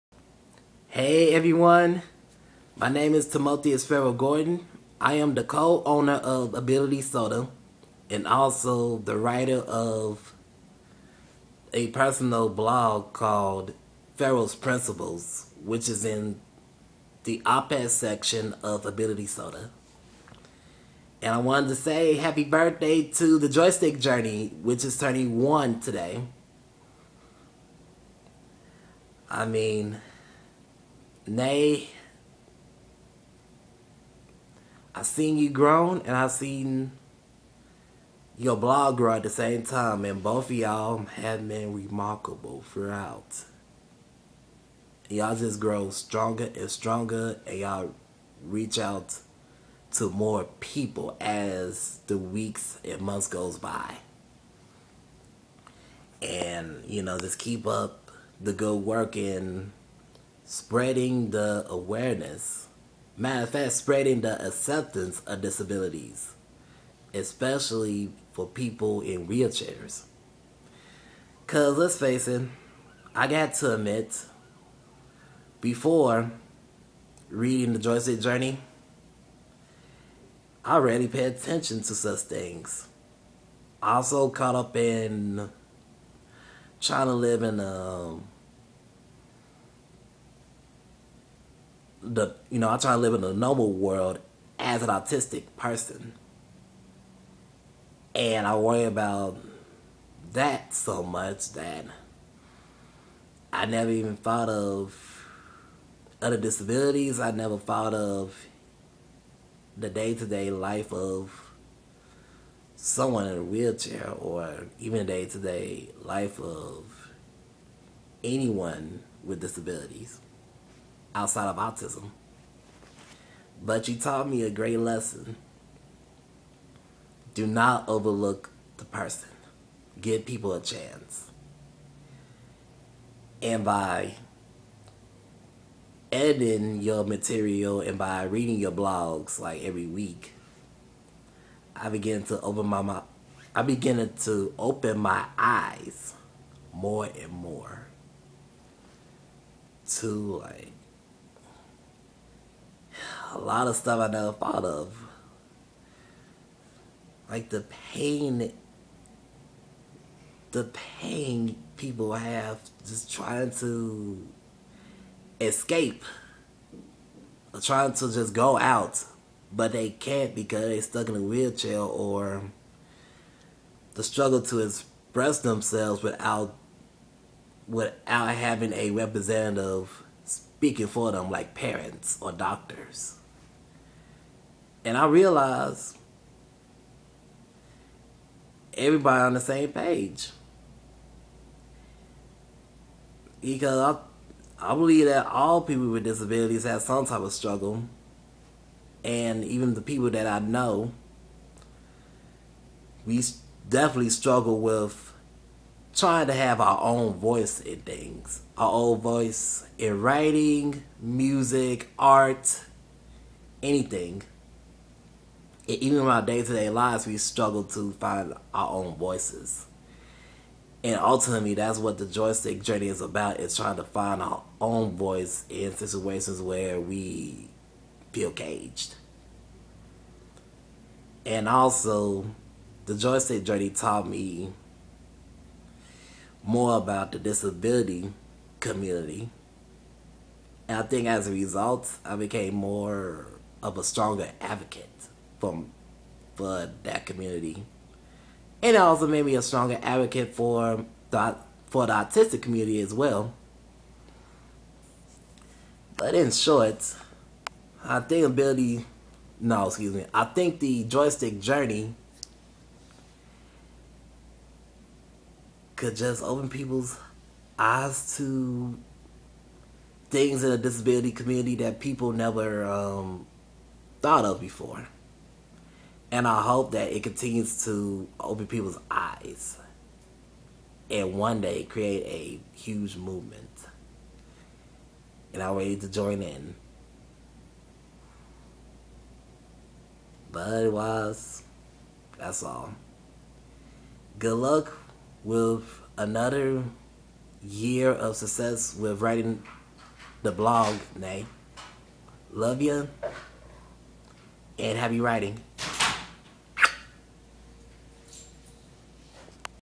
Happy 1st Birthday, Joystick Journey!! (For AbilitySota Co-Owner, Shanae Heard)
Joystick Journey turning 1 on 2/28 (Shanae's blog started on 2/29/2012)